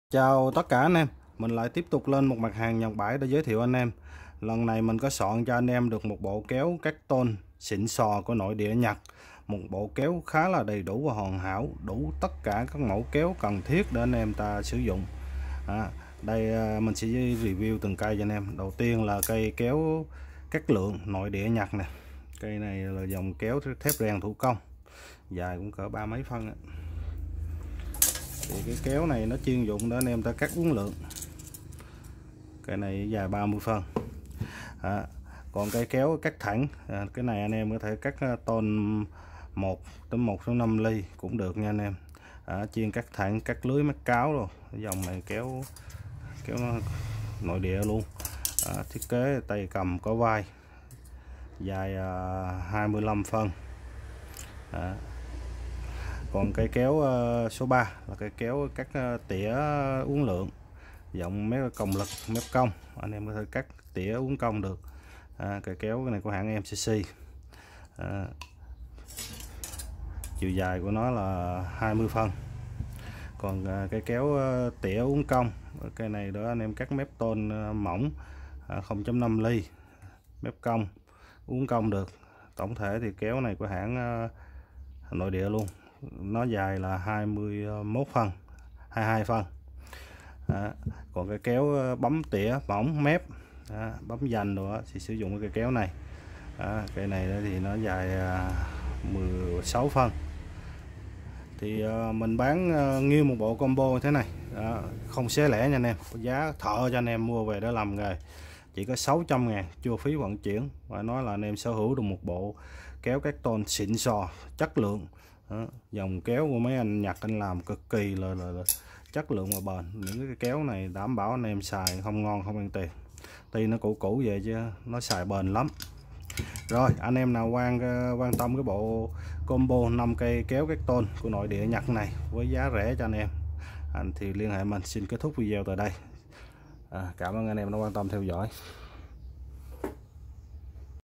combo 5 kéo cắt tôn nội địa Nhật Bản giá rẻ, chỉ 600k,chưa phí ship (đại 0966425154-zalo)
Soạn được 1 bộ combo 5 cây kéo cắt tôn nội địa Nhật Bản, đủ các loại mẫu kéo chuyên dụng để anh em cắt các loại tôn(kéo cắt thẳng, kéo uốn lượn,cắt mép ...)giá rẻ chỉ 600k,chưa phí ship. Chỉ có 1 bộ duy nhất. Anh em nhanh tay lh đại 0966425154-ship cod toàn quốc